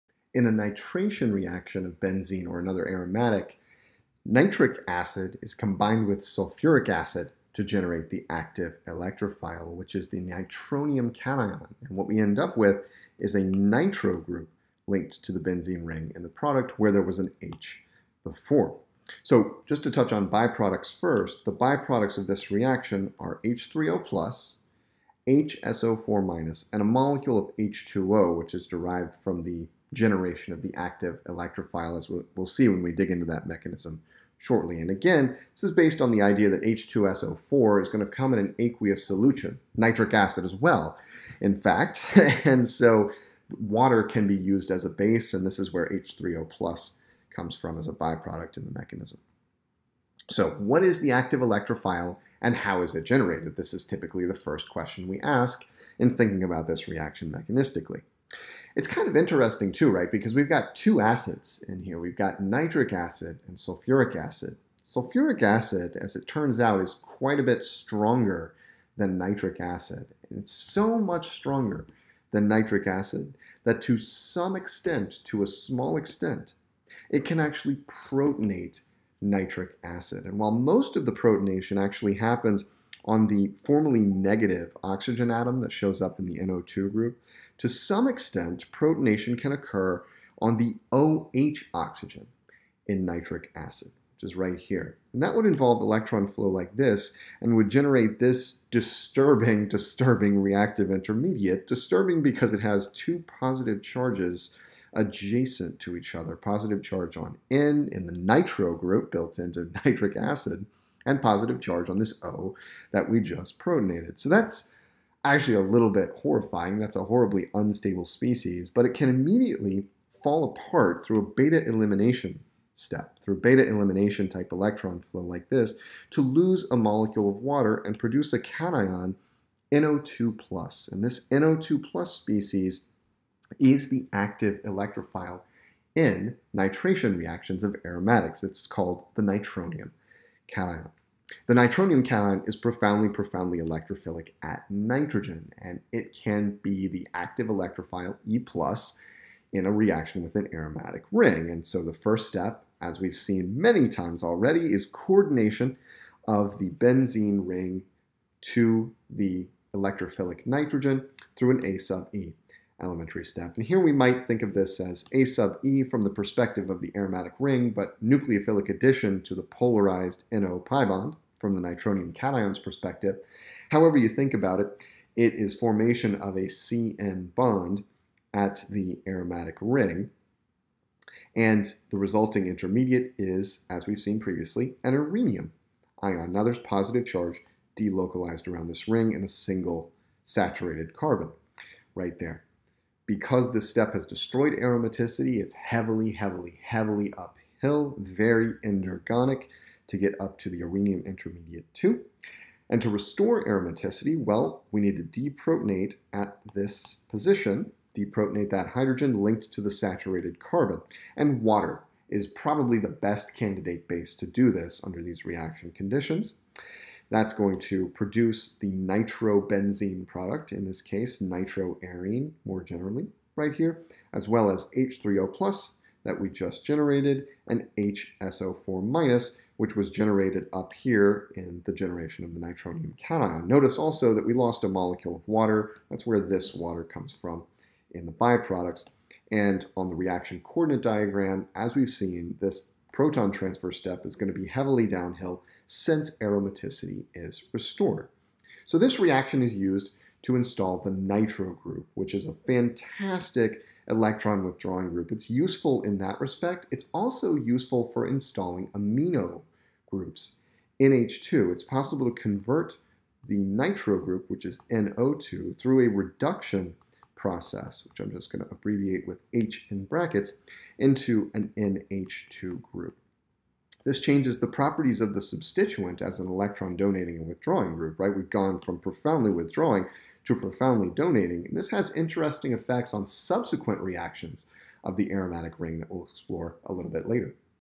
Nitration of Aromatic Compounds
00:00 Reagents, Products, and Byproducts
00:50 Generation of the Active Electrophile
02:21 The Standard EAS Mechanism in Nitrations
04:08 Reduction of Nitrobenzenes to Anilines